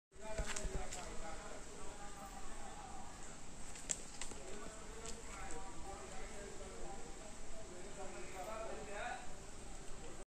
Monkey put bread in his leg finger. love all beings
Monkey put bread in his leg finger.